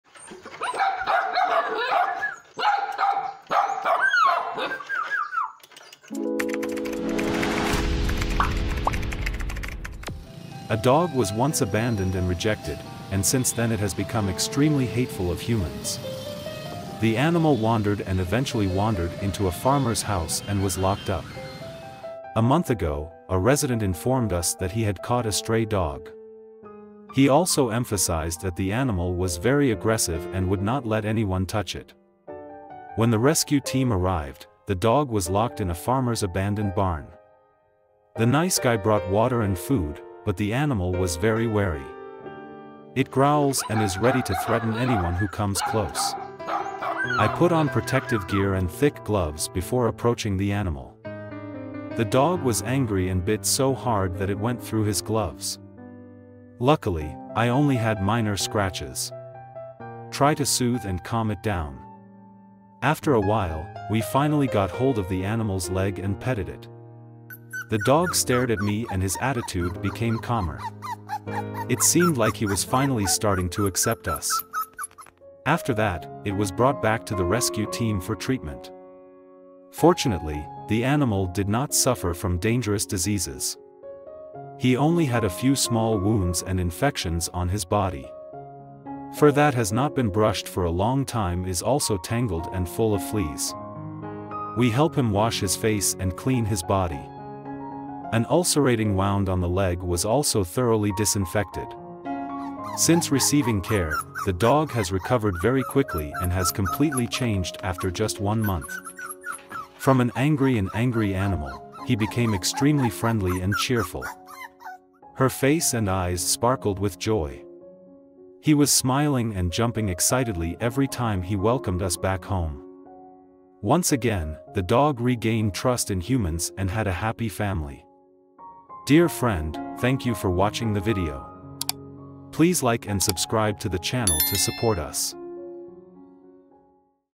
He acted aggressive when someone approached, but then the dog cried because...
A dog was once abandoned and rejected, and since then it has become extremely hateful of humans. The animal wandered and eventually wandered into a farmer's house and was locked up.